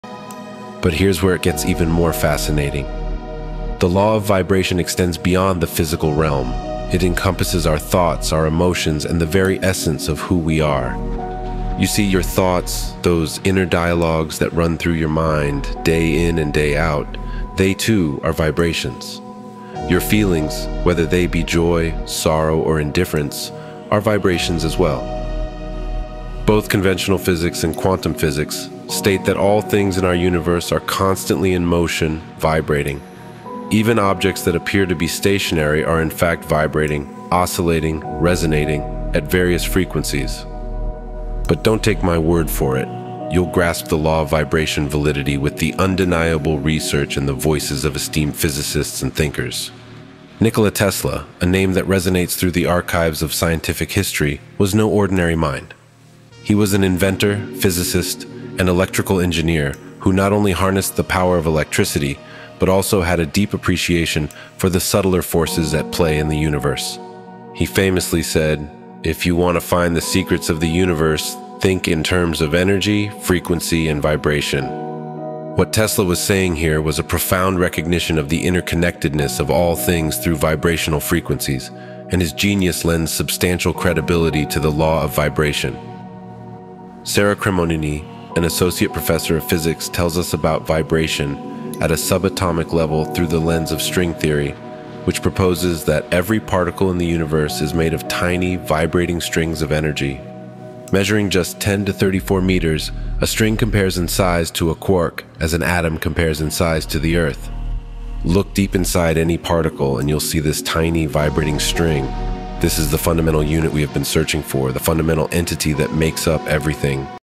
Law of Vibration: Tesla's Secrets to the Universe
Explore the fascinating law of vibration! We delve into how thoughts, emotions, and everything around us vibrates. Featuring insights from Nikola Tesla and Sarah Cremonini, we uncover the universe's secrets through energy, frequency, and vibration.